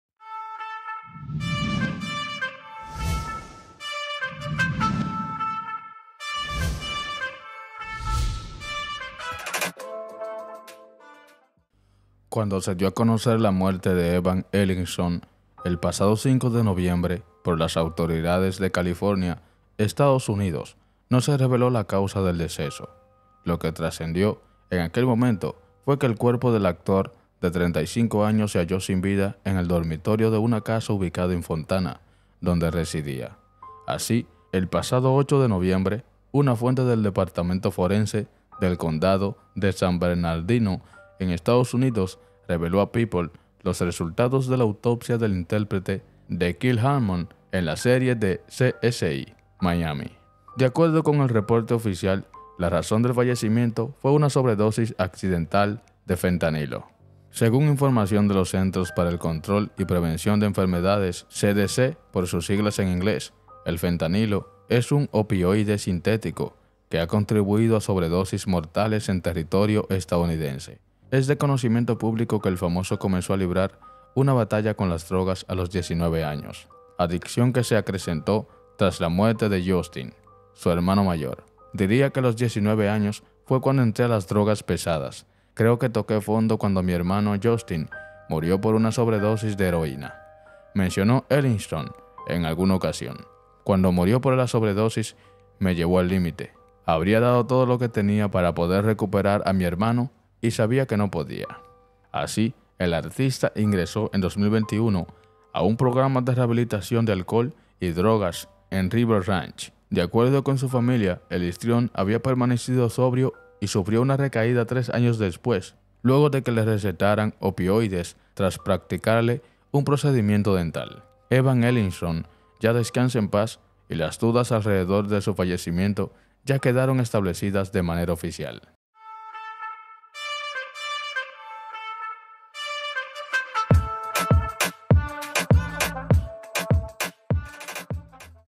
Se revelan las causas de la muerte del actor de 35 años, Evan Ellingson
A casi un mes del deceso del actor Evan Ellingson, se dan a conocer las razones de su fallecimiento de manera inesperada.